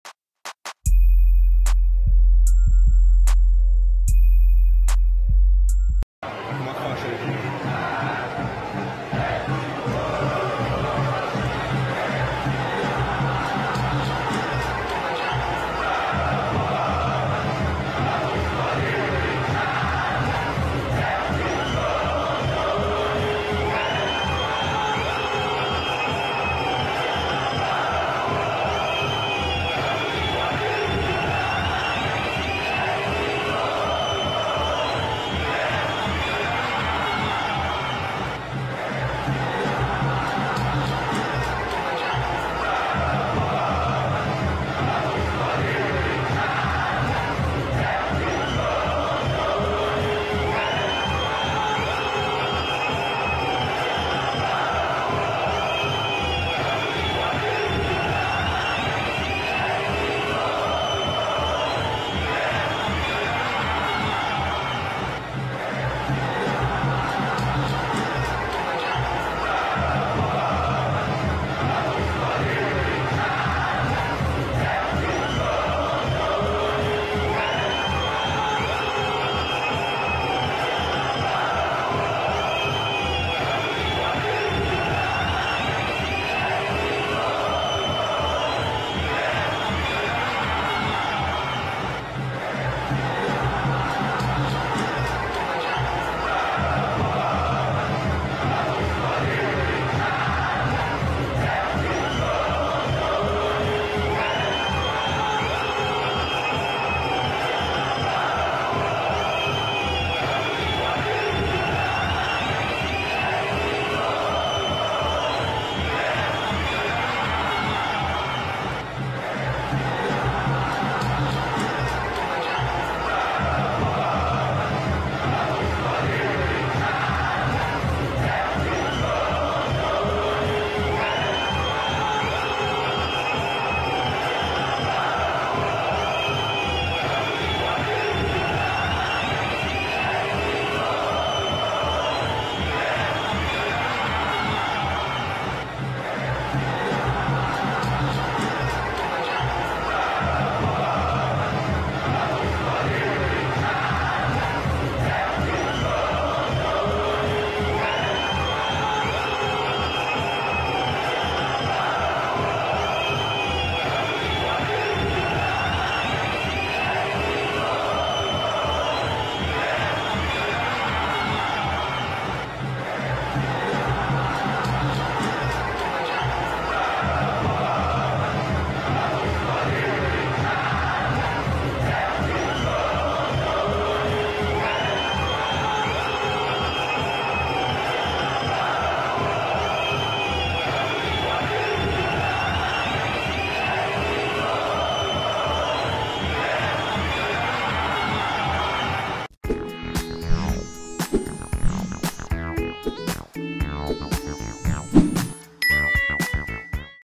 DE ARREPIAR A FESTA DA TORCIDA DO CORINTHIANS!!
Torcida do CORINTHIANS fazendo uma grande festa!